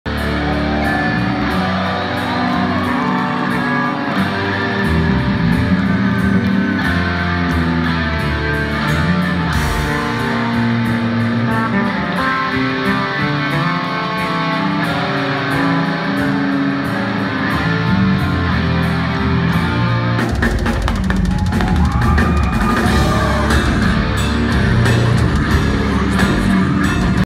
Throwback: When we played in Mexico 🇲🇽 with Obscura
When we played in Mexico 🇲🇽 with Obscura.
Mexico is one of my favourite places and I can’t wait to be back!